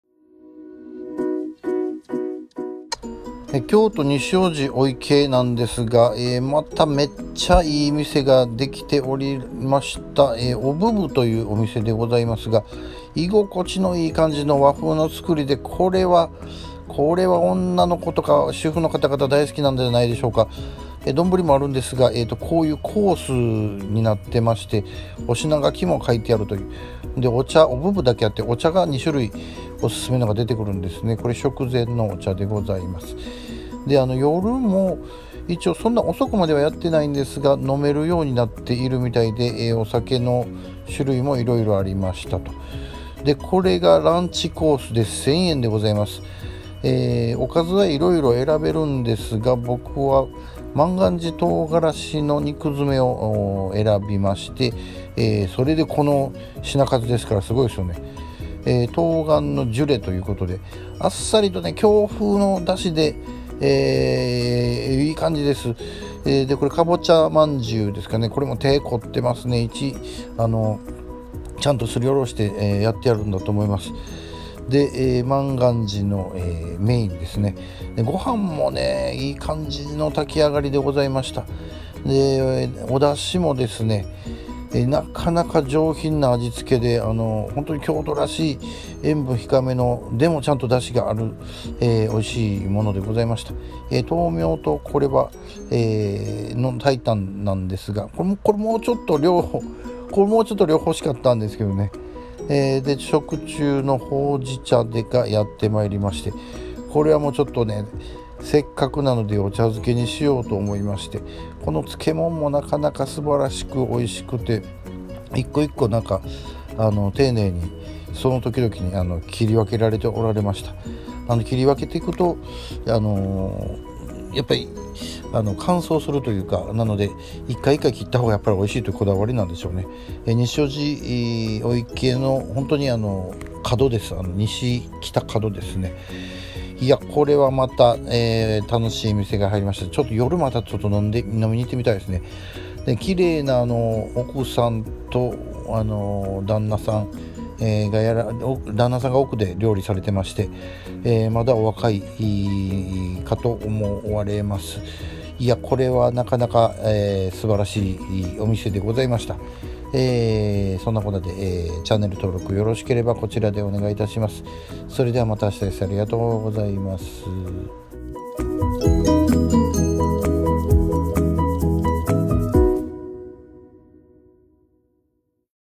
新店！ヘルシーおしゃれなランチコース！京都西大路御池「おぶぶ」
こんなところに新しい店がまたできてました。

居心地の良いおしゃれなヘルシーなお店でした(^○^)。